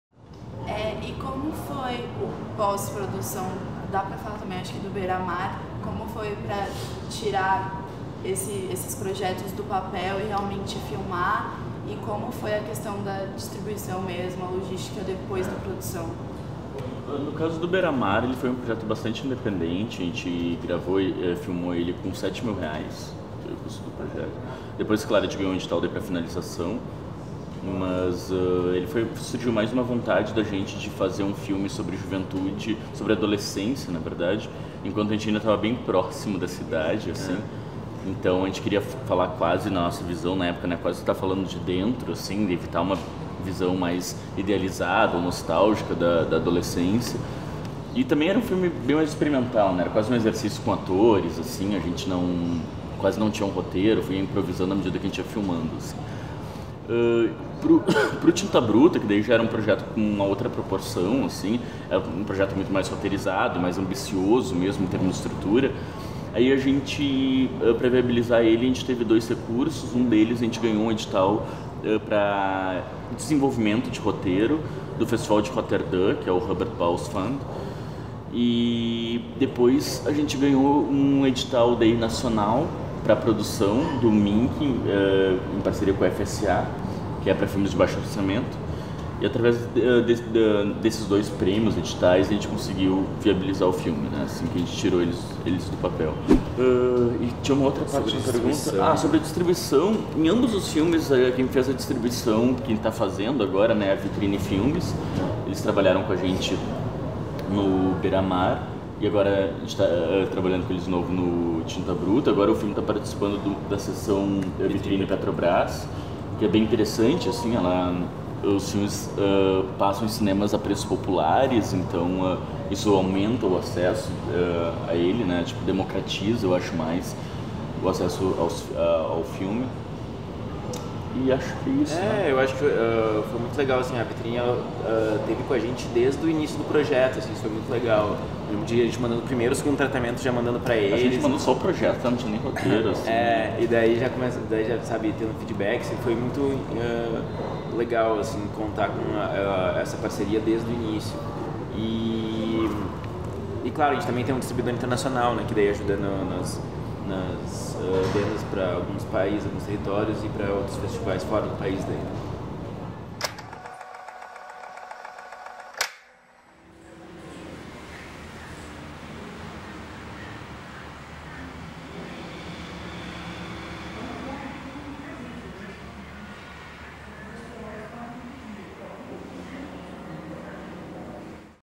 Pré-Produção, Produção, Pós-Produção, Distribuição | Filipe Matzembacher e Marcio Reolon
Filipe Matzembacher e Marcio Reolon nos falam das principais etapas de um FILME! Entre elas: Pré-Produção, Produção, Pós-Produção, Distribuição
Ambos são DIRETORES e ROTEIRISTAS. Confira o vídeo! ;-)

⦁ FILIPE MATZEMBACHER e MARCIO REOLON
Filipe Matzembacher e Marcio Reolon são de Porto Alegre, Rio Grande do Sul.
Ambos trabalham juntos nas etapas de ROTEIRO e de DIREÇÃO, e dessa maneira conjunta já criaram diversos filmes. Entre eles os longa-metragens BEIRA-MAR e TINTA BRUTA.

⦁ INSTITUTO DE CINEMA (INC)